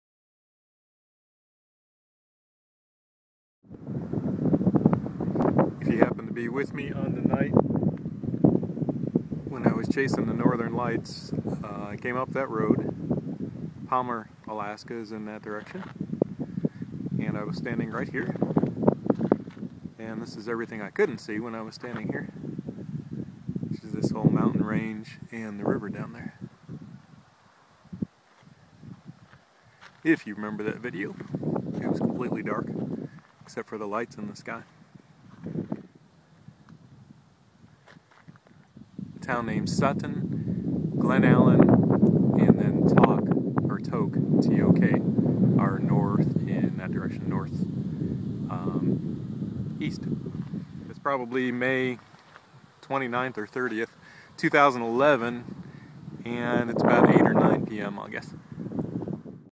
Glenn Highway, North of Palmer, Alaska
On the Glenn Highway, about twenty miles north of Palmer, Alaska, on the Glenn Highway, standing in the same place where I watched the Northern Lights this winter.